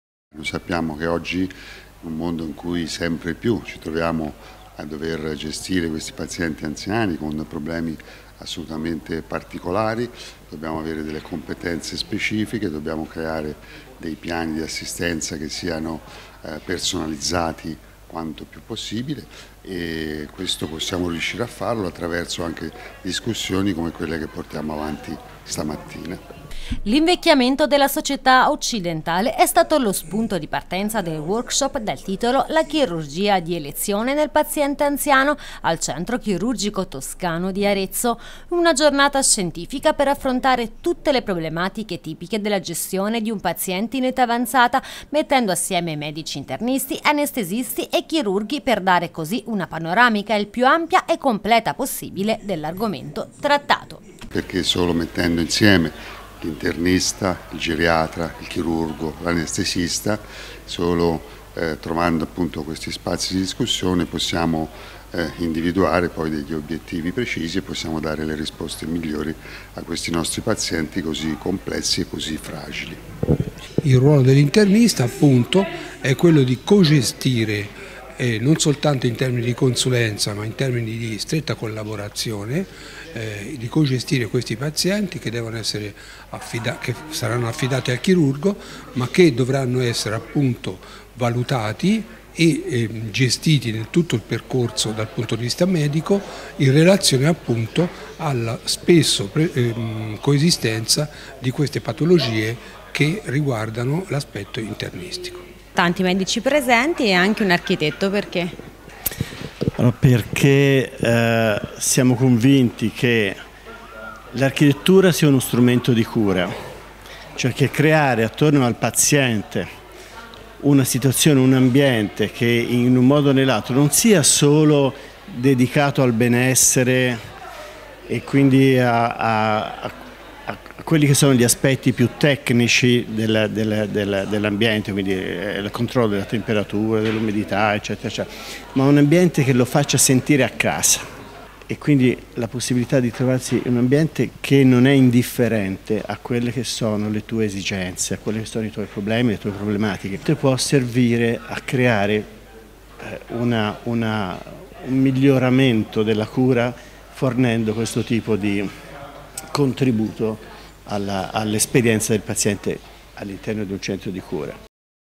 "La chirurgia di elezione nel paziente anziano". Il workshop al Centro Chirurgico Toscano
L’invecchiamento della società occidentale è stato lo spunto di partenza del workshop dal titolo “La chirurgia di elezione nel paziente anziano”  al Centro Chirurgico Toscano.
Una giornata scientifica per affrontare tutte le problematiche tipiche della gestione di un paziente in età avanzata, che mette insieme medici internisti, anestesisti e chirurghi per dare una panoramica il più ampia e completa possibile all’argomento trattato.